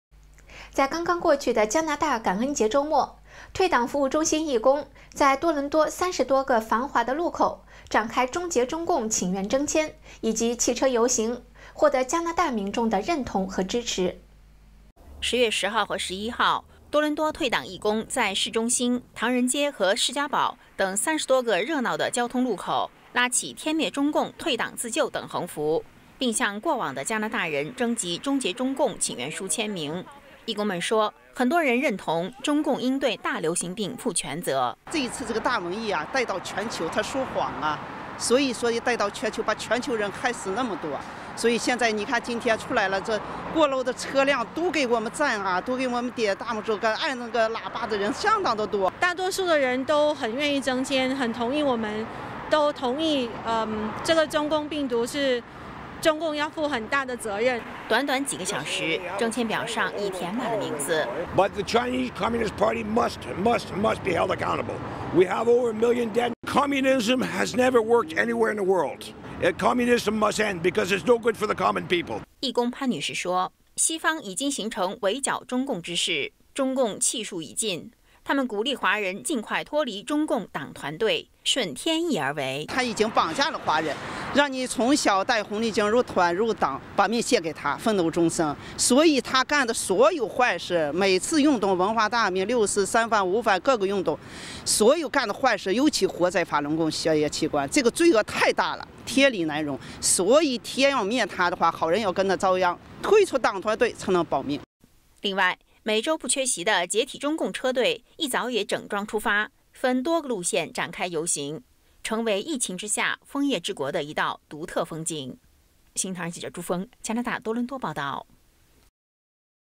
退党热潮 加拿大民众支持终结中共【三退】
【新唐人北京时间2020年10月13日讯】刚刚过去的加拿大感恩节周末，退党服务中心义工在多伦多30多个繁华路口展开「终结中共」请愿征签以及汽车游行，获得加拿大民众认同和支持。

10月10日和11日，多伦多退党义工们在市中心、唐人街和士嘉堡等30多个热闹的交通路口拉起「天灭中共退党自救」等横幅，并向过往的加拿大人征集「终结中共」请愿书签名。义工们说，很多人认同中共应对大流行病负全责。

退党义工 潘女士：「这个大瘟疫啊带到全球，它（中共）说谎啊，把全球人害死那么多。今天出来的过路的车辆都给我们赞，竖大拇指，按喇叭的人相当的多。」

退党义工 张女士：大多数人都很愿意征签（签名），都同意这个中共病毒中共要负很大的责任。

短短几个小时征签表上已写满了名字。

加拿大民众 Pierre Lauzon：「中共必须为这场瘟疫负全责，上百万人已经失生命。共产主义在任何地方都行不通。共产党必须终结，因为它不为百姓谋福利。」

义工潘女士说，西方已经形成围剿中共之势，中共气数已尽，他们鼓励华人尽快脱离中共党团队，顺天意而为。

退党义工 潘女士：「它（中共）已经绑架了华人，让你从小戴红领巾入团入党，把命献给它奋斗终身，所以它干的所有坏事，每次运动文化大革命、六四、三反五反，尤其是活摘法轮功学员器官，这个罪恶太大了， 天理难容，所以天要灭它的话，好人要跟着遭殃，退出党团队才能保命。」

另外，每周不缺席的「解体中共」车队一早也整装出发，分多个路线展开游行，成为疫情之下多伦多的一道独特风景。

新唐人记者朱峰加拿大多伦多报导